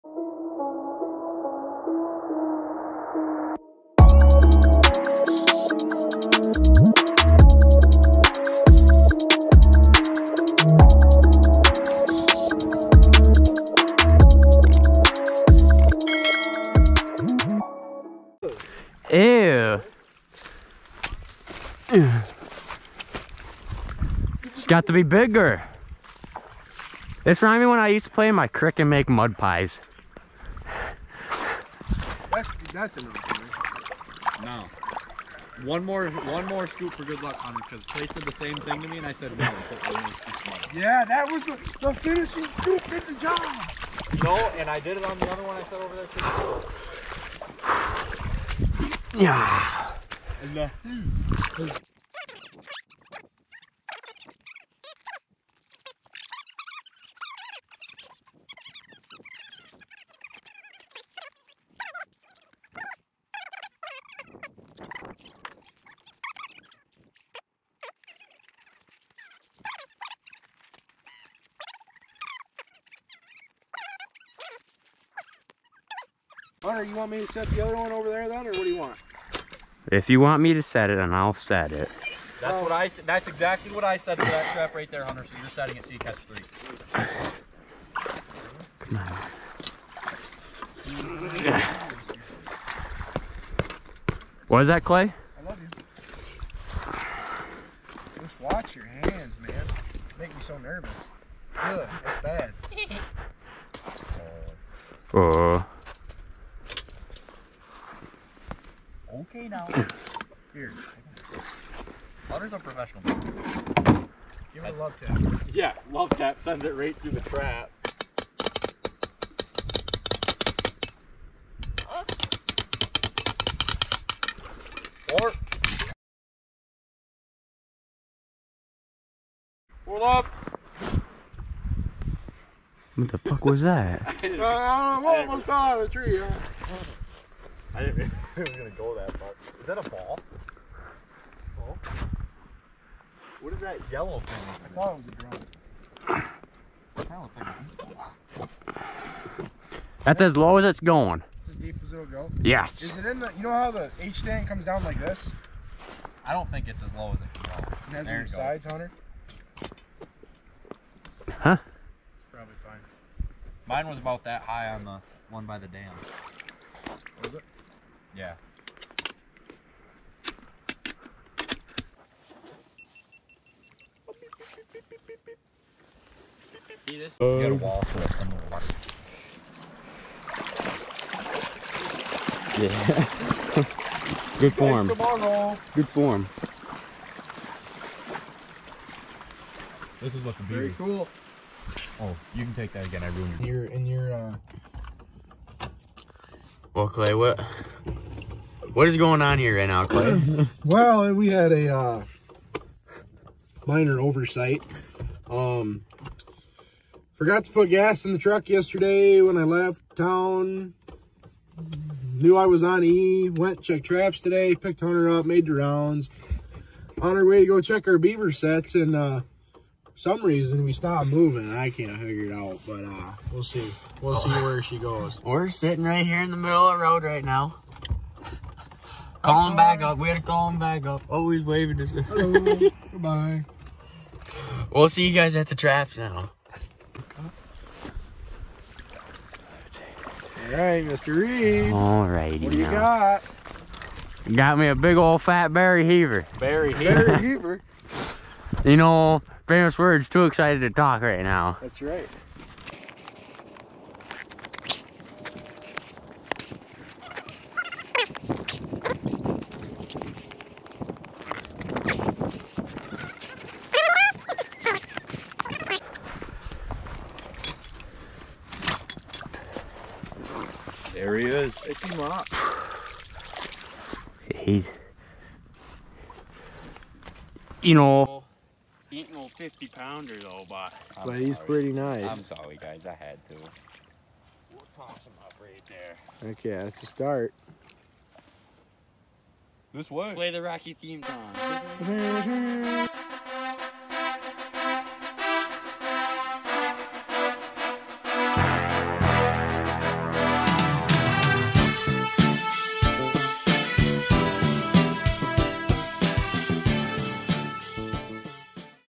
Michigan Beaver Trapping |First Ever Beaver
In todays video Hunter goes out to set traps for a beaver. He ends up getting his first beaver, hope you guys enjoy.